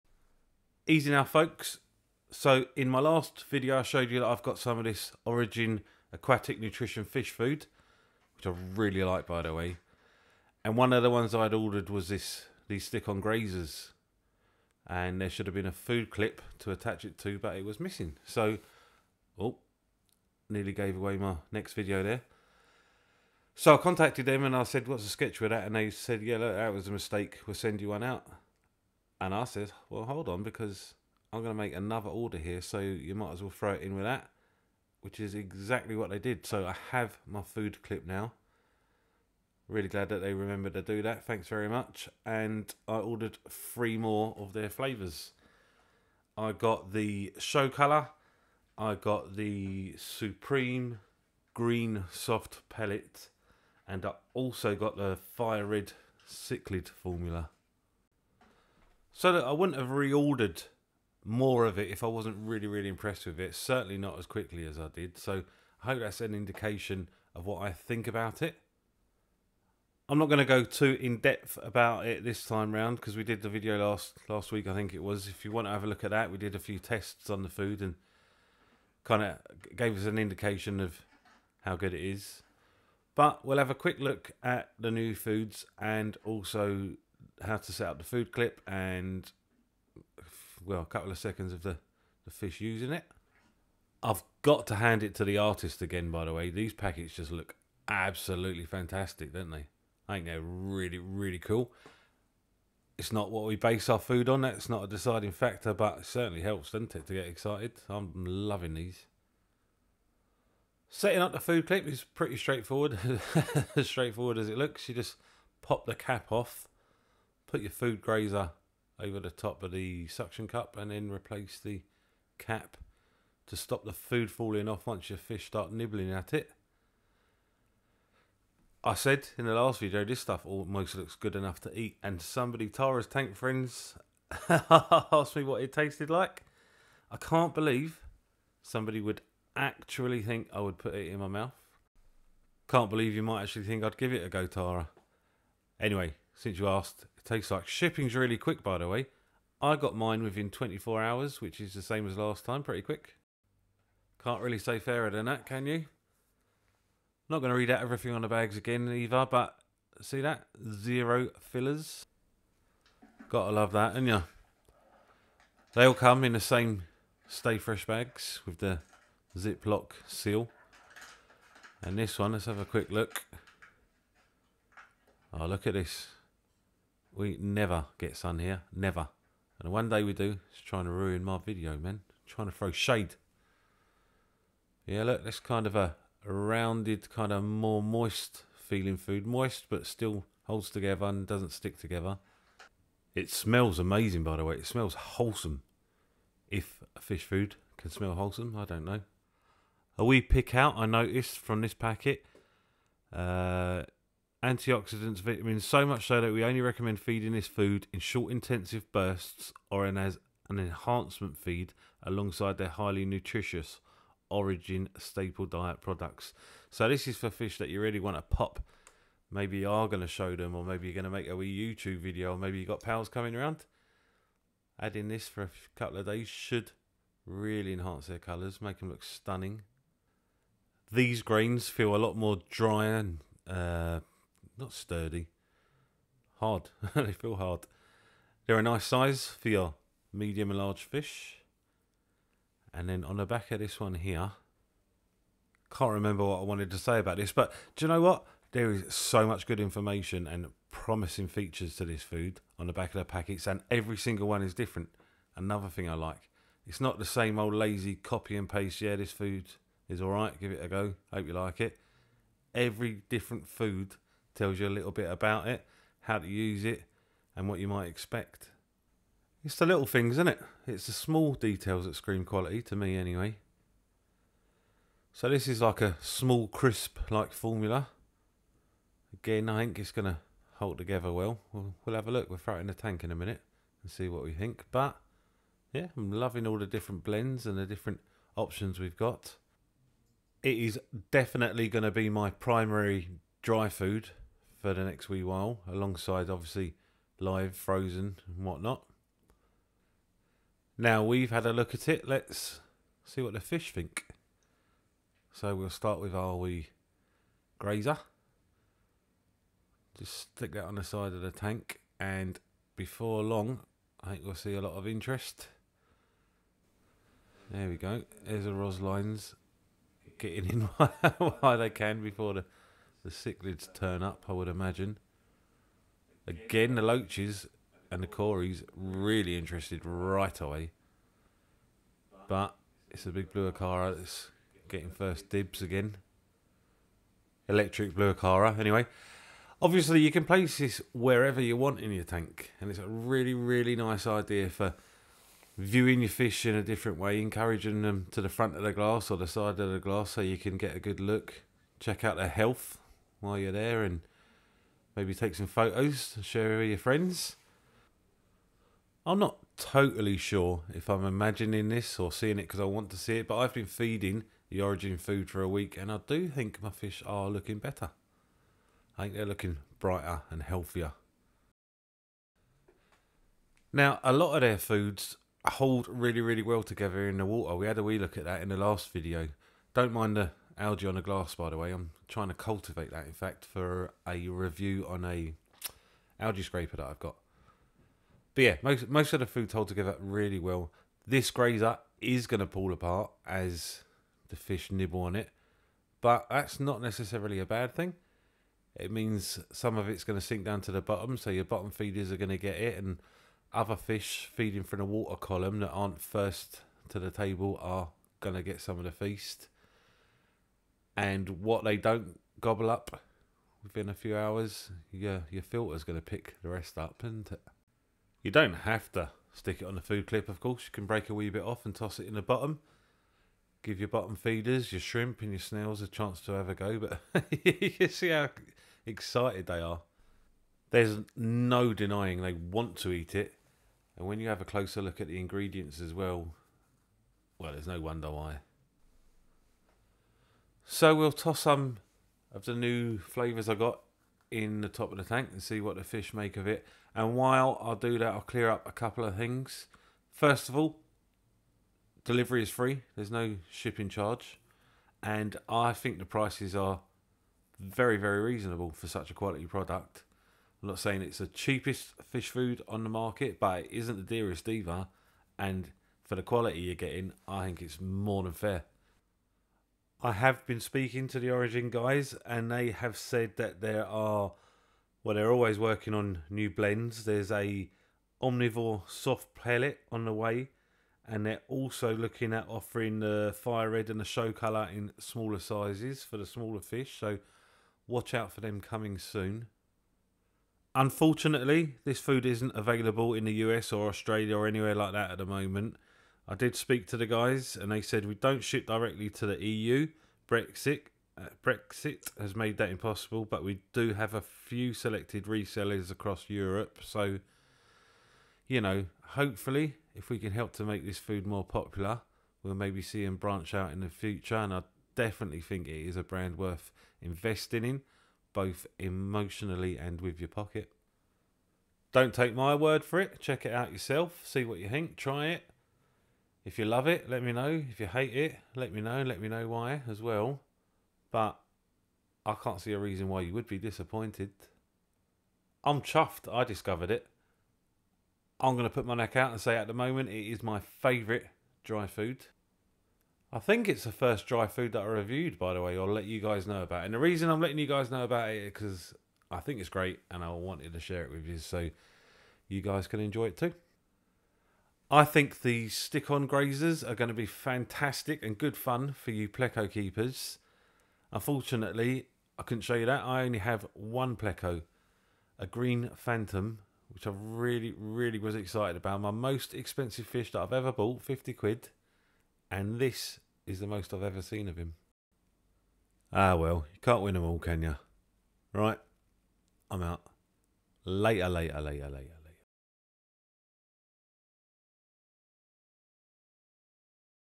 ORIGIN FISH FOOD - MORE IN THE RANGE AND THE FEEDING CLIP
I got the feeding clip delivered and tried out more in their range.